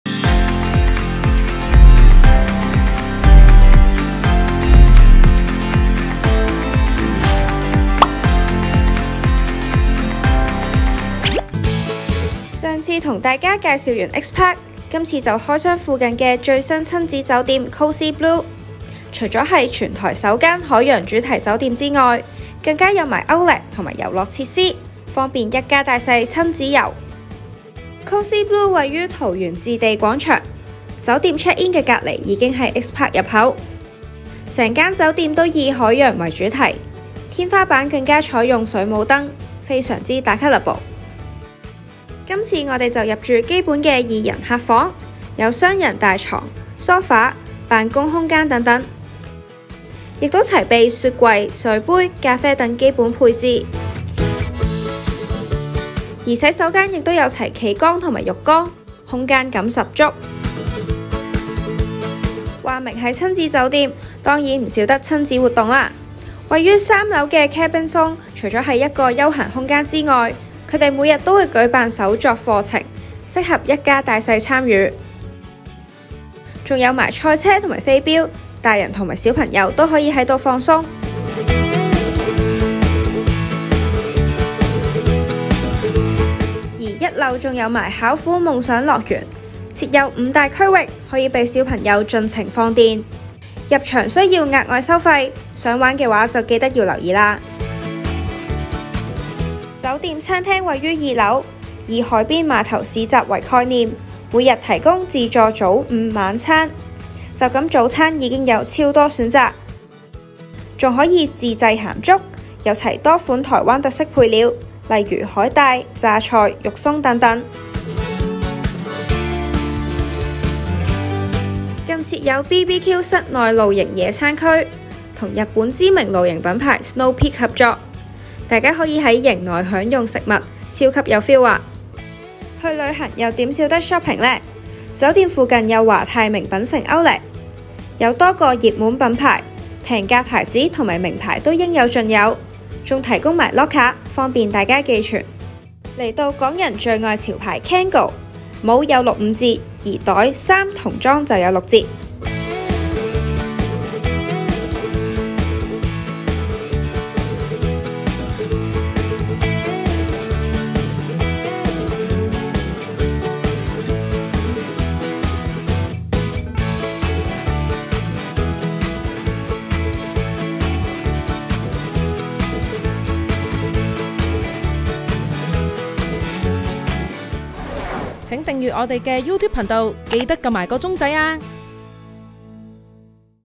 台灣自由行｜開箱桃園最新親子酒店COZZI Blu全台首間海洋主題/離Outlet 5分鐘距離
上次跟大家介紹完桃園水族館X-Park，今次就開箱附近的最新親子酒店COZZI Blu。除了是全台首間海洋主題酒店之外，更有齊Outlet及遊樂設施，方便一家大小親子遊。

更正：片中酒店名稱為COZZI Blu

鳴謝：臺灣觀光協會香港辦事處 長榮航空
採訪：Kristy 攝影：Kristy

COZZI Blu
地址：桃園市中壢區春德路101號
COZZI Blu位於桃園置地廣場，為和逸飯店旗下酒店之一，更是全台首間以海洋為主題的五星級酒店。酒店地理位置方便，旁邊就是置地廣場有Xpark水族館、新光影城、華泰名品城outlet。整間酒店都以海洋為主題，牆身以深藍色為主，一踏走酒店如同走進海洋世界一般。其中電梯位置的天花板設有大型水母燈飾，非常打卡able。

bmgproductionmusic